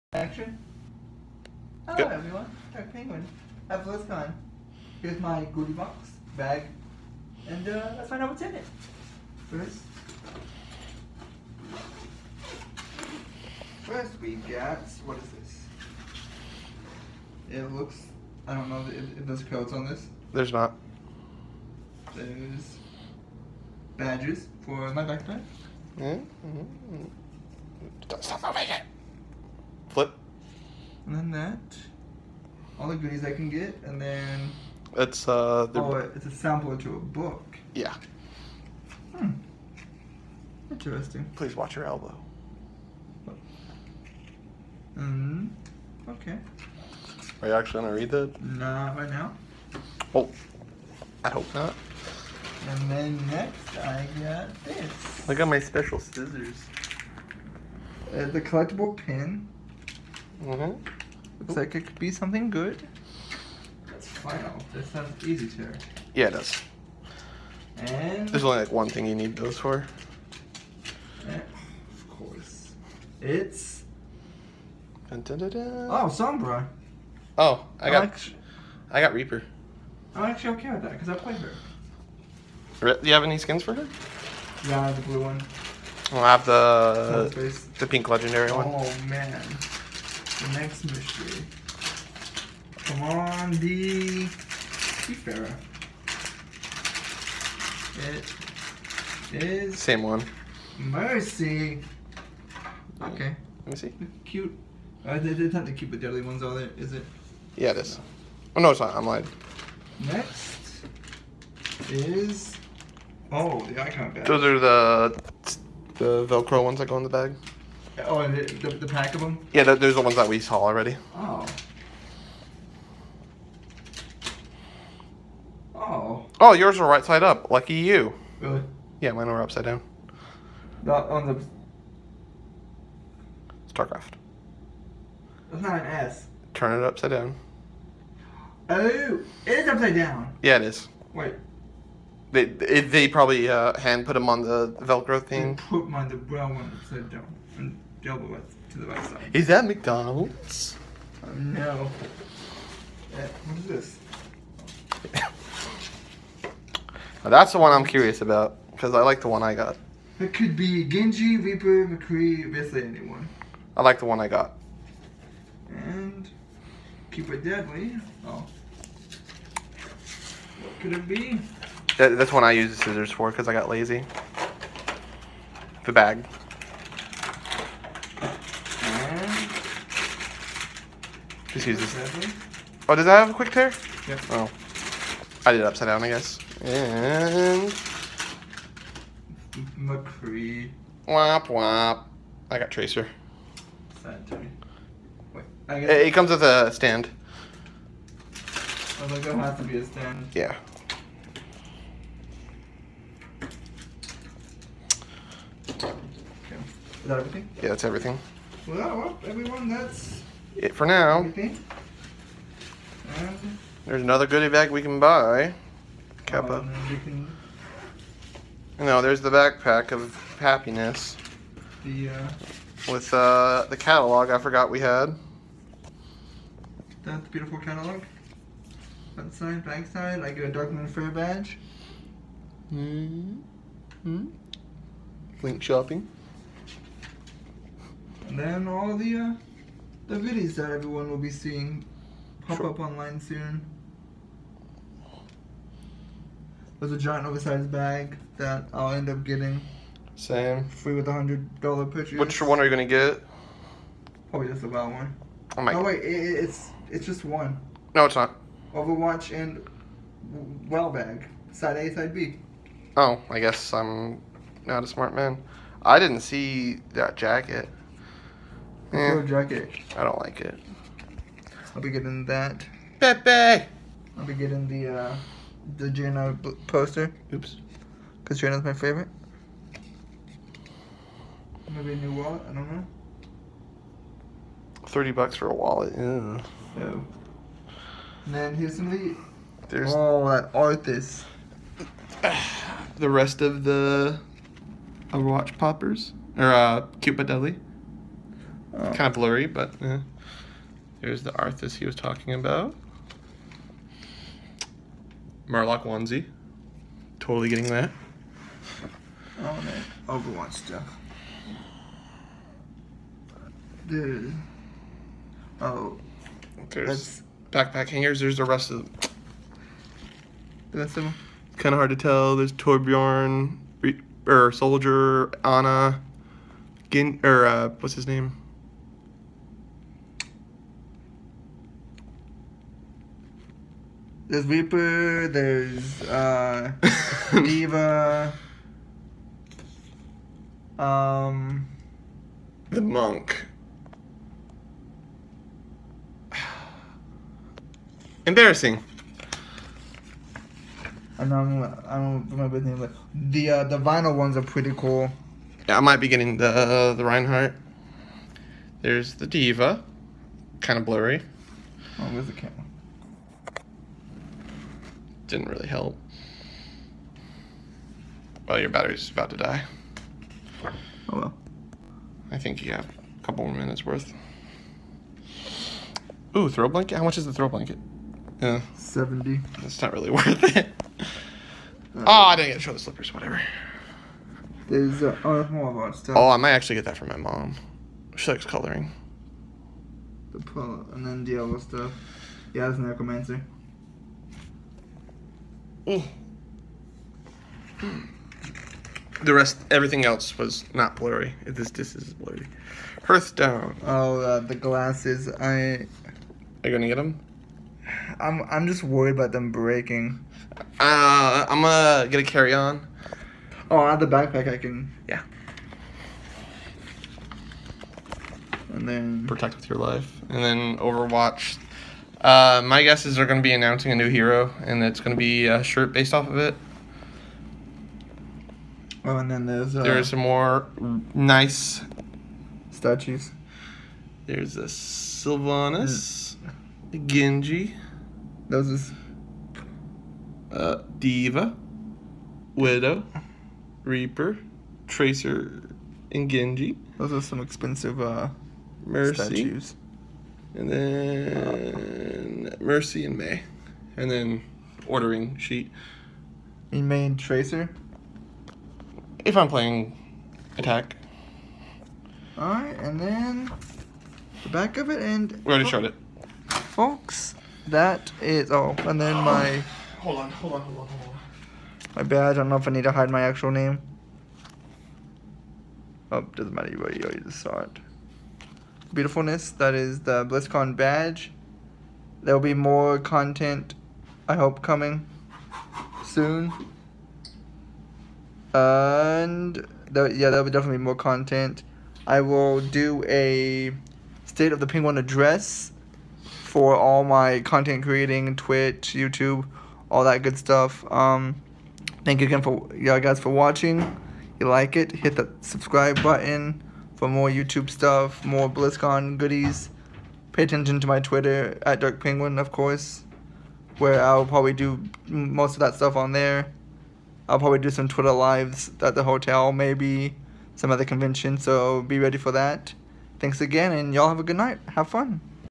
Blizzcon 2017 Goody Bag
The opening of the Blizzcon Goody bag 2017